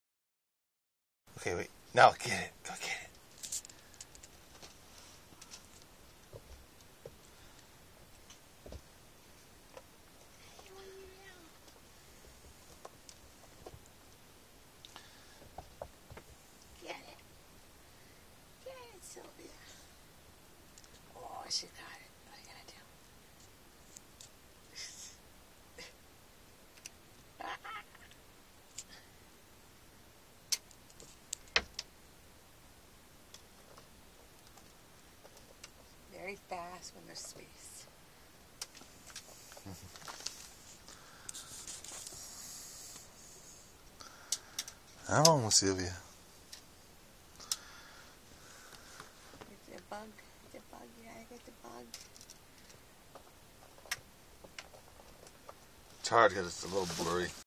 Sylvia playing with her favorite toy.
This wonderful tortoiseshell kitten is looking for a forever home.  Obviously we are fostering her, but we cannot keep forever since there is a limit on how many animals we can have.